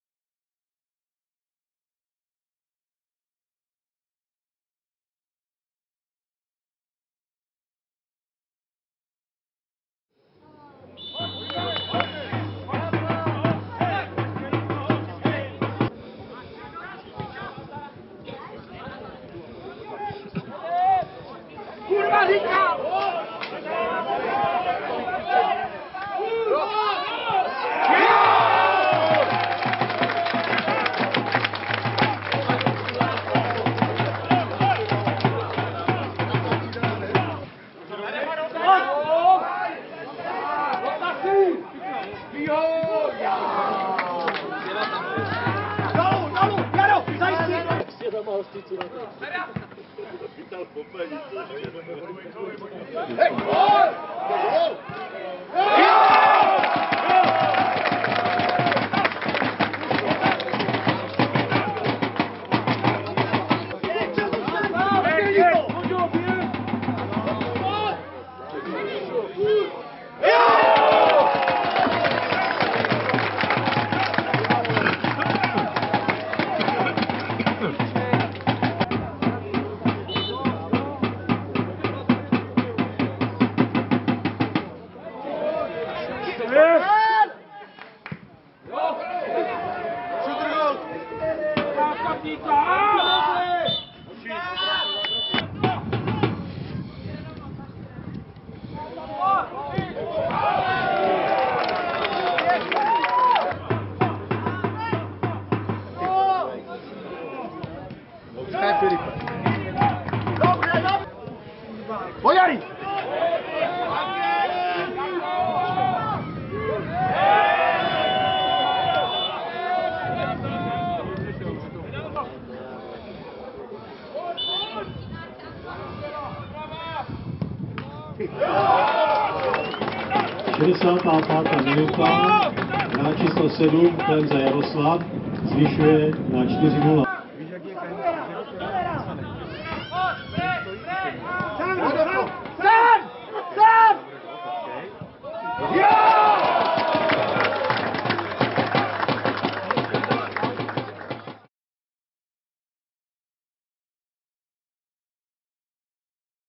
Rapotín - Litovel 5:0 (3:0)
V jasnou záležitost proměnili hráči Rapotína domácí souboj s Litovlí. Už po pěti minutách hry se tým ze Šumperska radoval z prvního gólu, do přestávky přidali Rapotínští ještě dva přesné zásahy. Tatran se alespoň o kosmetickou úpravu výsledku snažil marně, i ve druhé půli se prosazovali jen domácí. Z penalty navýšil Klemsa a na konečných 5:0 upravil Drozd.